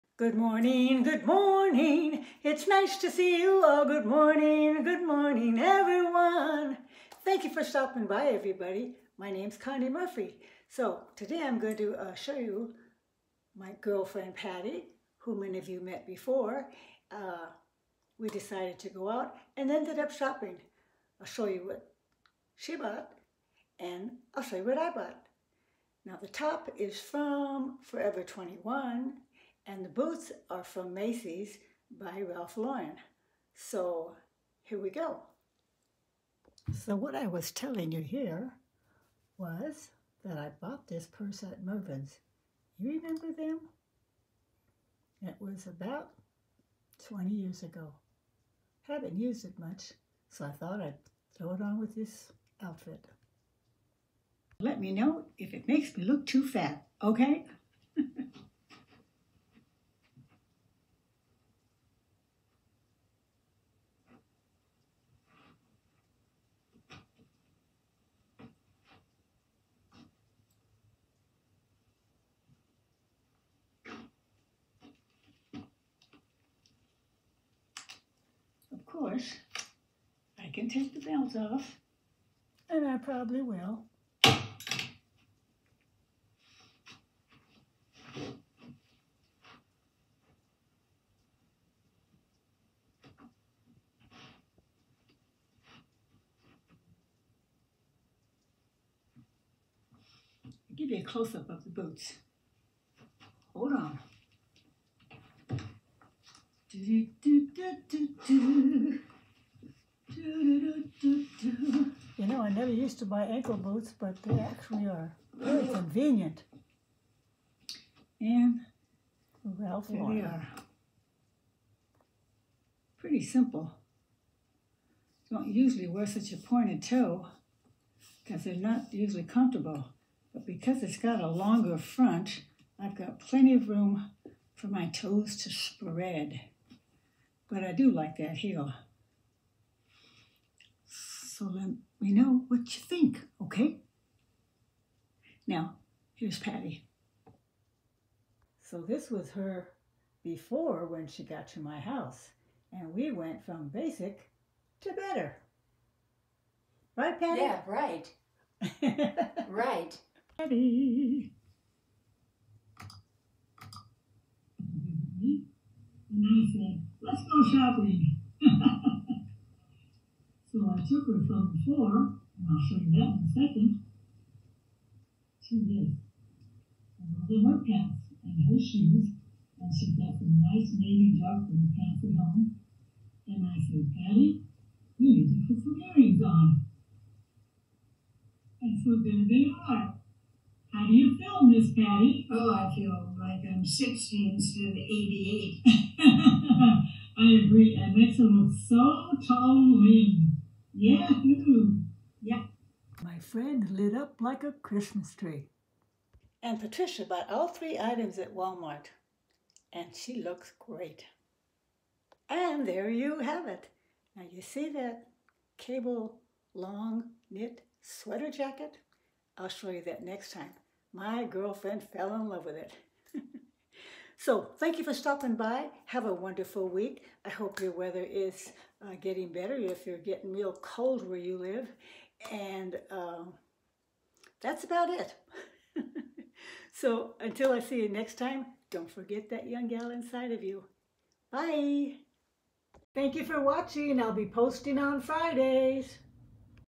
Before and After picture. SHE went from "88 to 55 yrs.young and looked so slim in new outfit
GIRLFRIEND PATTY was so excited after elevating her style of outfit and
 I will will model 2 items from my last haul.    hugs Connie